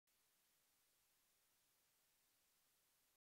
Dania Ramirez at Variety's 4th Annual Power Of Women Even...
Dania Ramirez at Variety's 4th Annual Power Of Women Event Presented By Lifetime on 10/5/12 in Los Angeles, CA